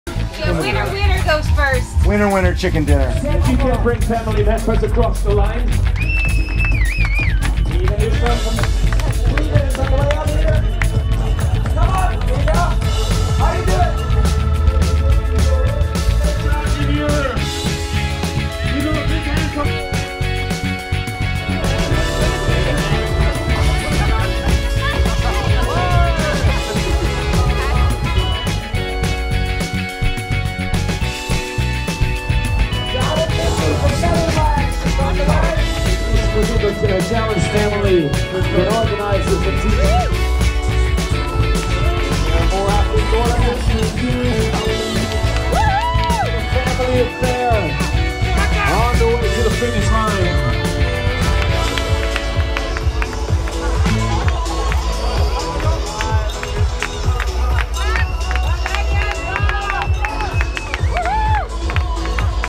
WINNER-WINNER-CHICKEN-DINNER
Some very happy faces are crossing the line at this years Challenge Denmark. It is fun and everyone here is a champion.

Remember